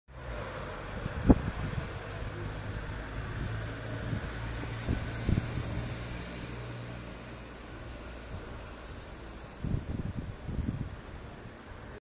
Hirtelen hóesés Szombathelyen
Rövid ideig óriási hóesés és szélvihar volt Szombathelyen, majd mintha mi sem történt volna, elállt.